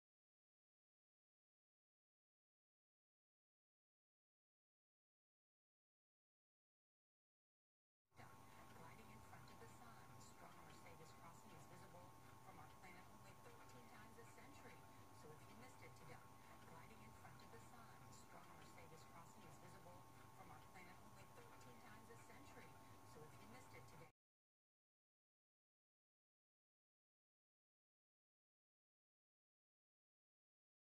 Mercury seen moving across the Sun!
This only can be seen about 13 times a century. Mercury, the smallest planet in our solar system, and only 35 million miles from the Sun, revolves around the sun every 88 days.